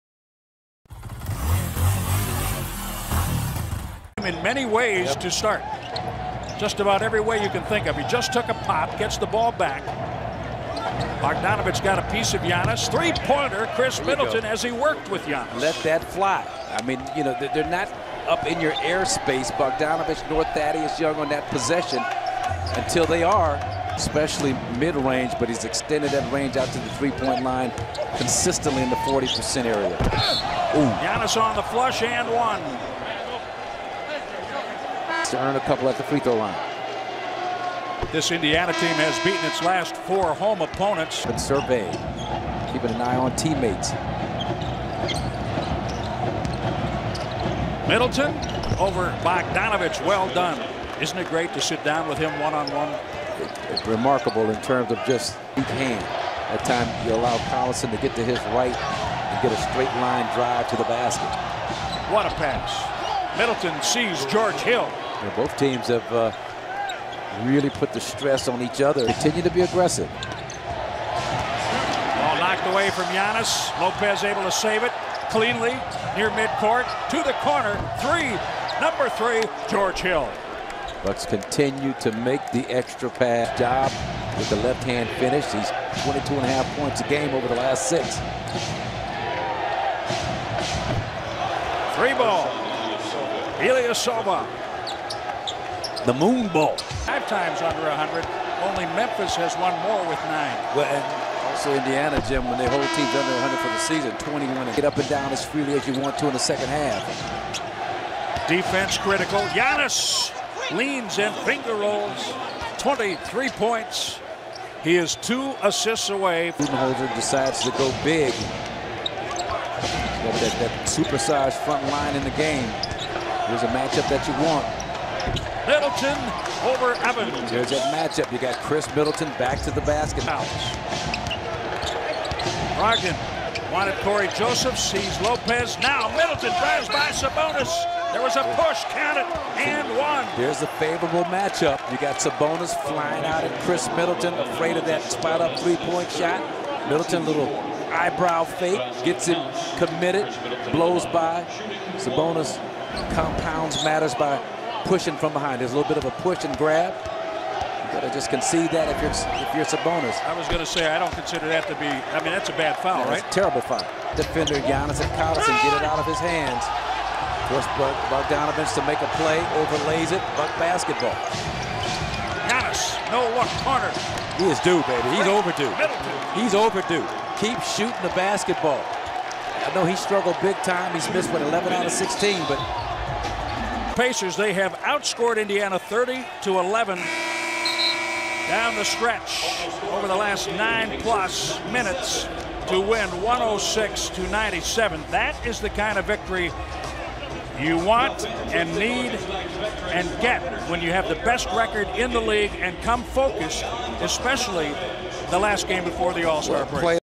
Khris Middleton Highlights vs Pacers RS19G57 - 15 Pts, 6 Rebs, 5 Asts (13.02.19)
NBA 2019 Regular Season Game 57 Milwaukee Bucks vs Indiana Pacers (Feb 13, 2019)